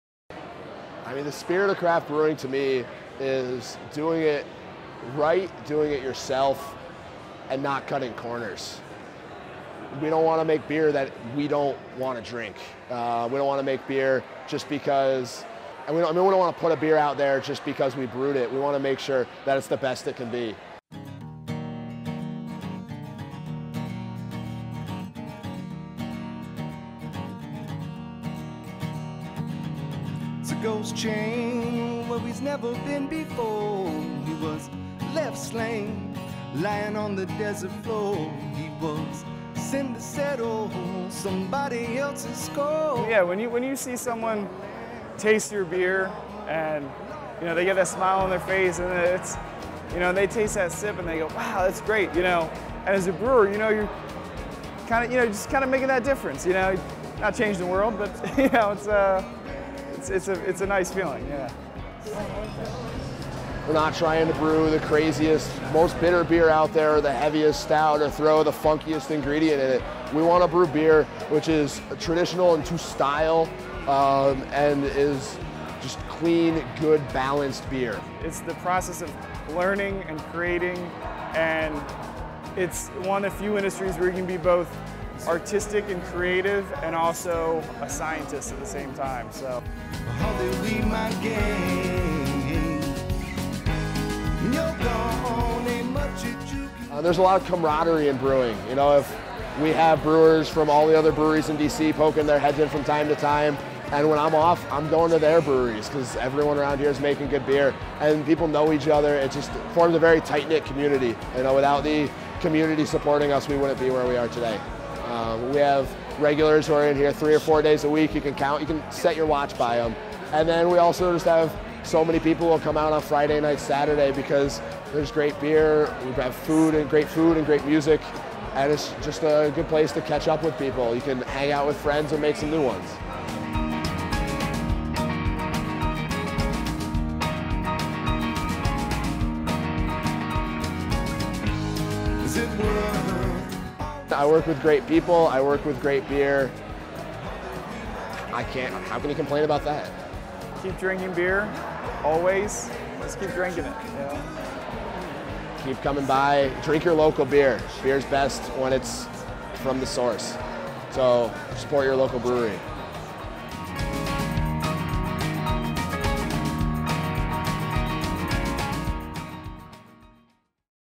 The Spirit of Craft Brewing
Huge thanks to Port City for letting my buddy and I film there over the weekend. We were attending a 4-day video production workshop for DOD and Govt. employees on improving the craft. We were given the theme "Spirit" and had to produce a very short film in less than 24 hours. This is what we came up with. "The Spirt of Craft Brewing"